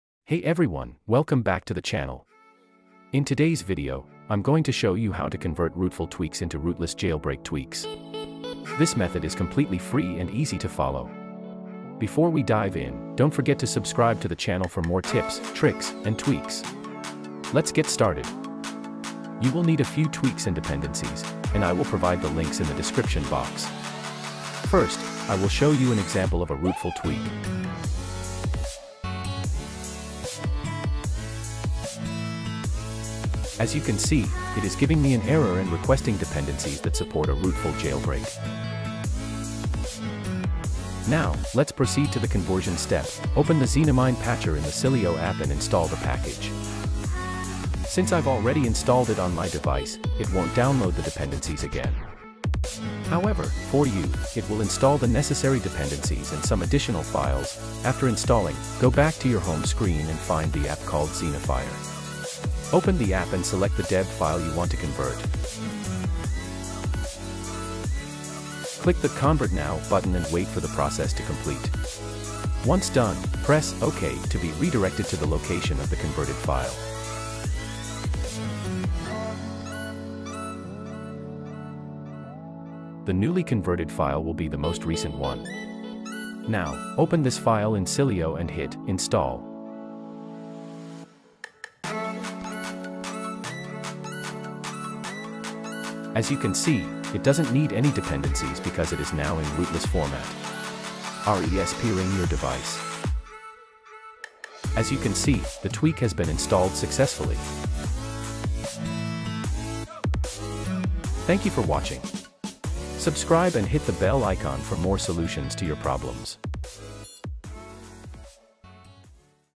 How to convert Rootful jailbreak tweaks into Rootless jailbreak | dopamine 2 jailbreak ios 15 16
Hello Everyone, Welcome to Next Solution.
I shared tips, tutorials and tech solutions on this channel.
Most of my content is about iOS jailbreak.

Links